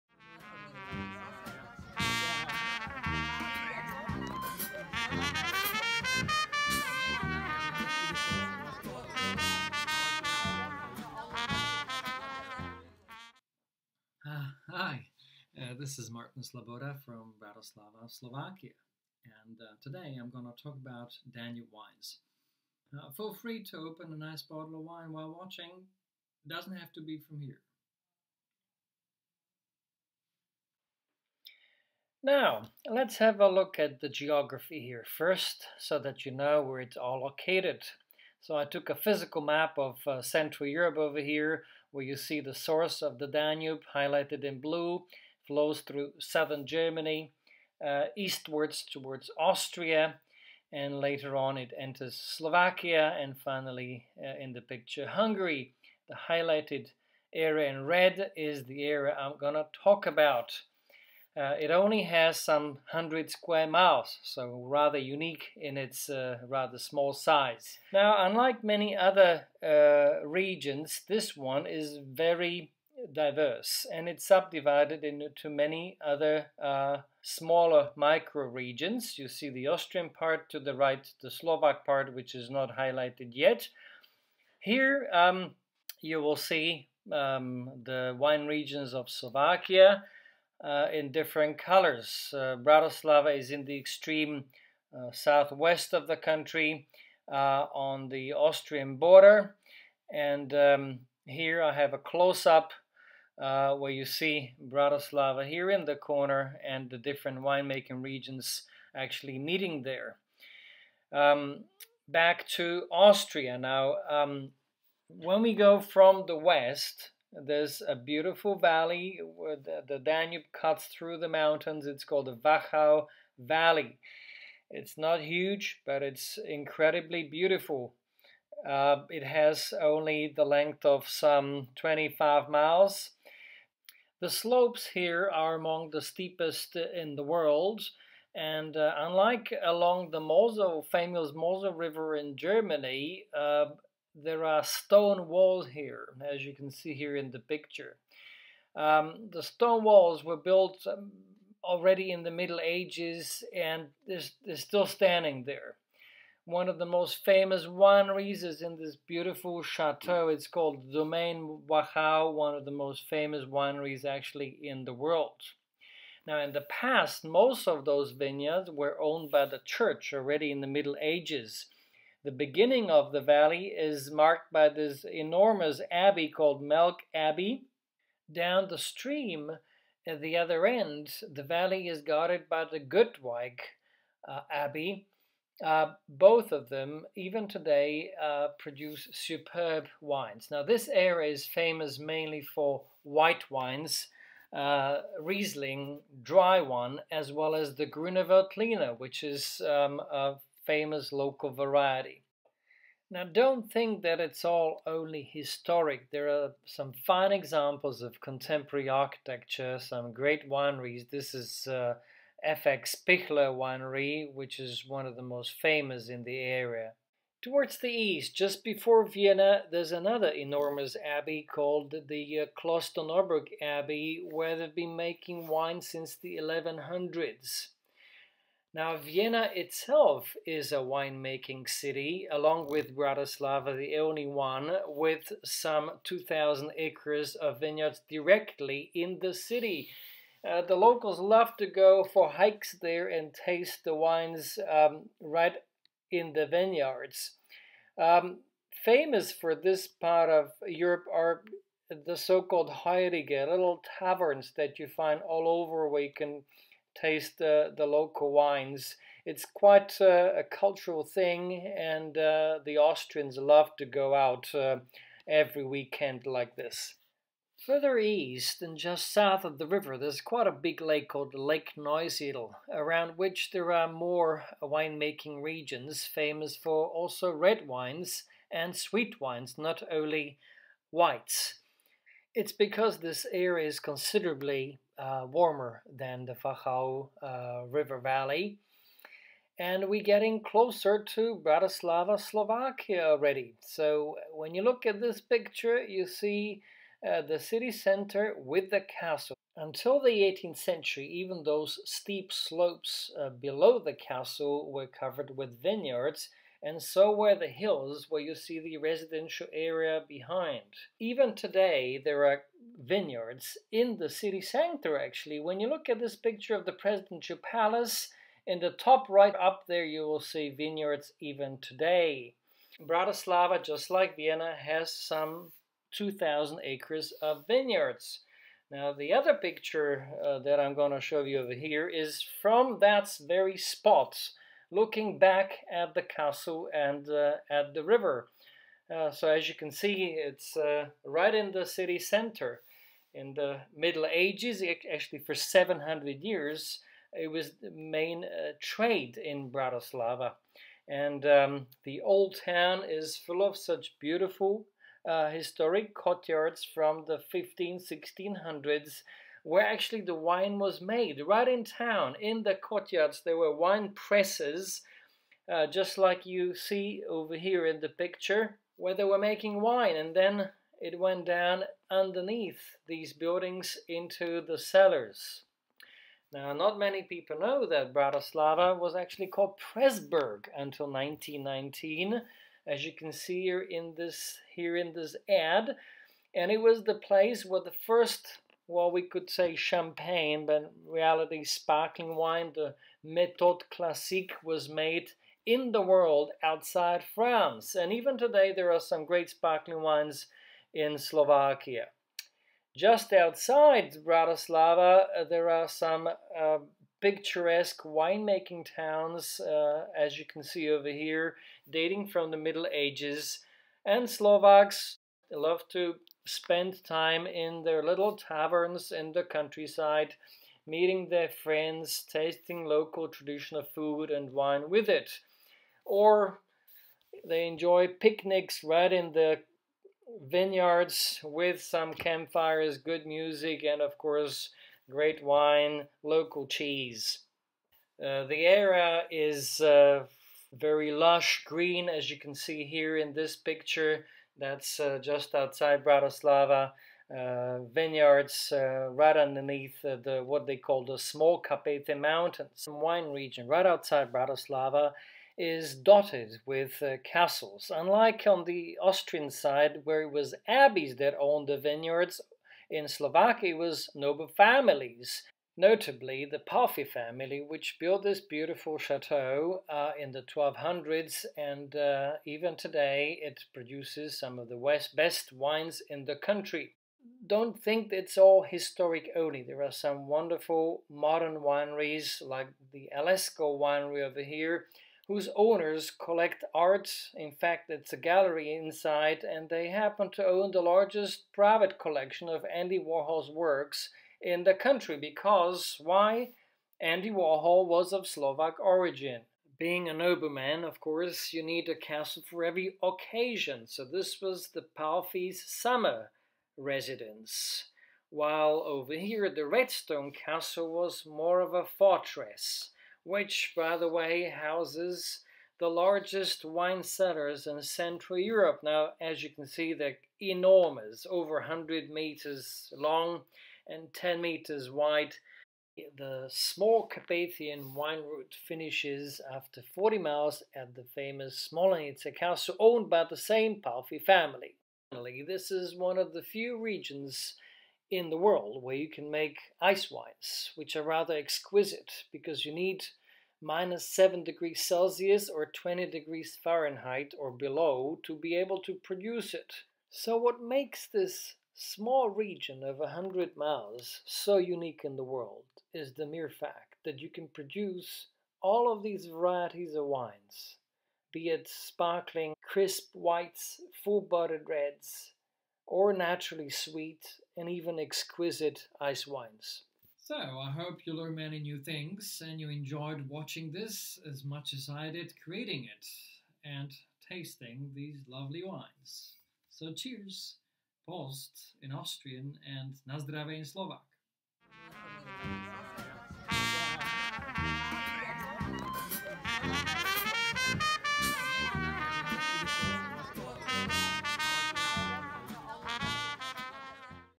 Danube Wines
Talk about the wine regions around the two closest capitals in Europe (Vienna, Austria and Bratislava, Slovakia), which are the only capitals with notable vineyards and wine production in the world as well as a unique diversity of various types of wines. For unique tailor-made wine tours in Slovakia